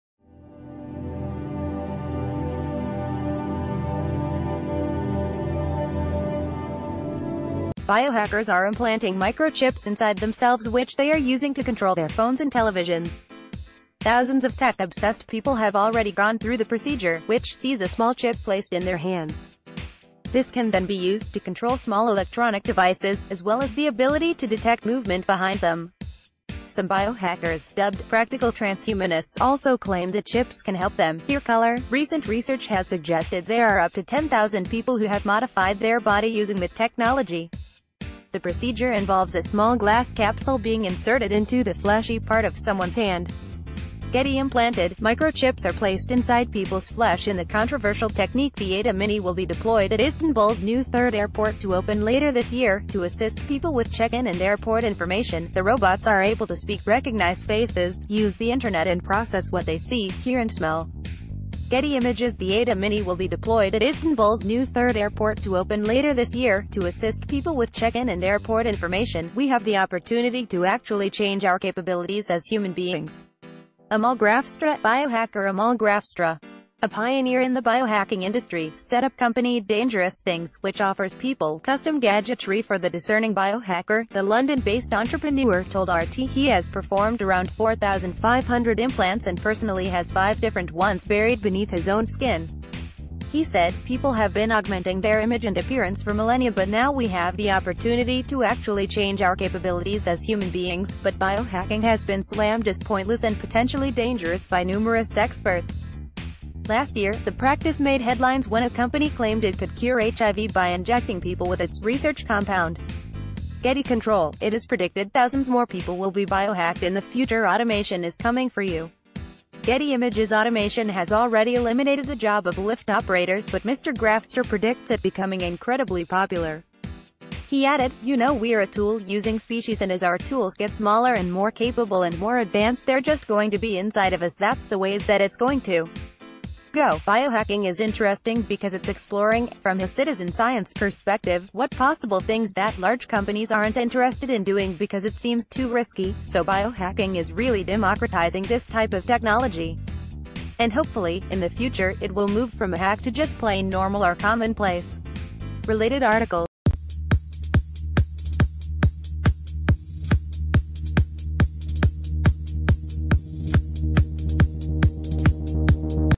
Breaking News | Biohackers are implanting MICROCHIPS inside skin to control TV and phones
BIOHACKERS are implanting microchips inside themselves which they are using to control their phones and televisions. Thousands of tech-obsessed people have already gone through the procedure, which sees a small chip placed in their hands. This can then be used to control small electronic devices, as well as the ability to detect movement behind them. Some biohackers – dubbed “practical transhumanists” – also claim the chips can help them “hear colour”.Recent research has suggested there are up t...